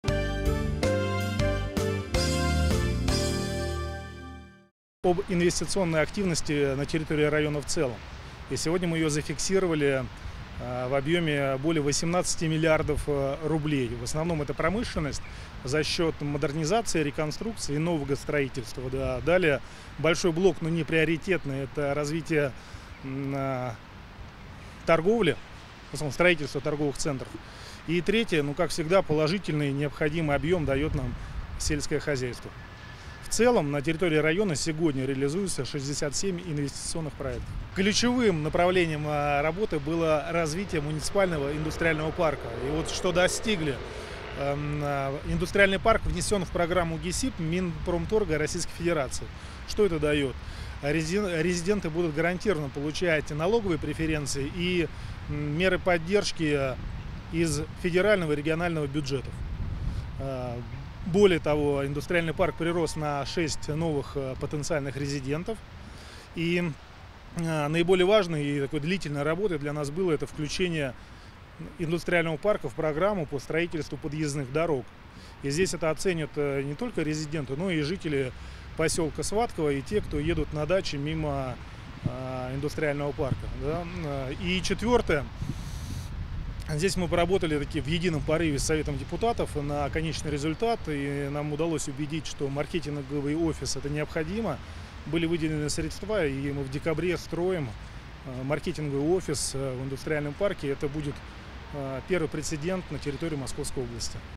Андрей Толстиков: «67 инвестиционных проектов реализуются в районе»
12 декабря 2017 | Инвестиционная активность в Сергиево-Посадском районе зафиксирована в размере 18 млрд рублей. На территории района реализуются 67 инвестиционных проектов. Ключевое направление – развитие муниципального индустриального парка «М-8», сообщает Андрей Толстиков, начальник управления инвестиций администрации Сергиево-Посадского района